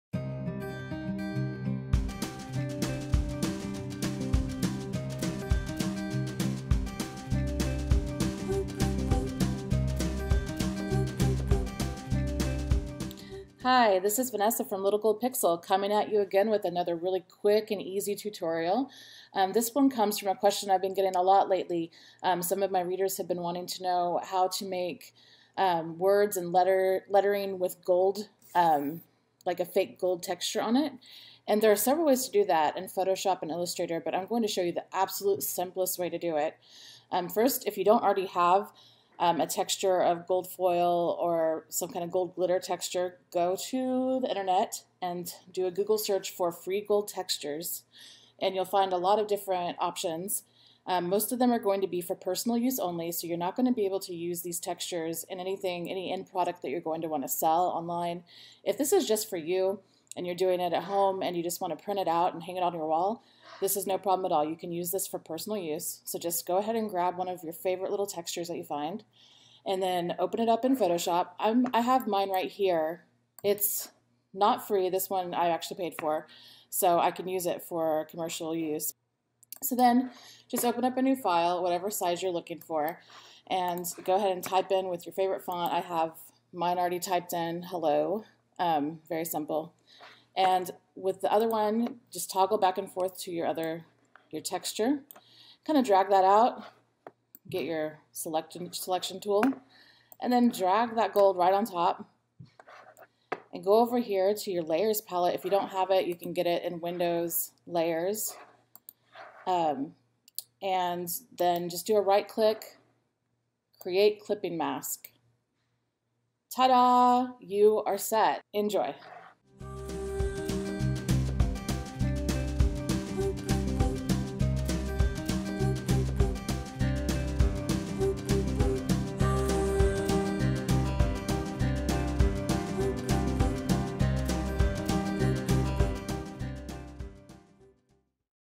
How to Add Gold Foil Effect to Your Text in Photoshop
This quick lesson shows you the fastest way to give your text that gold-foil effect in Photoshop. Enjoy!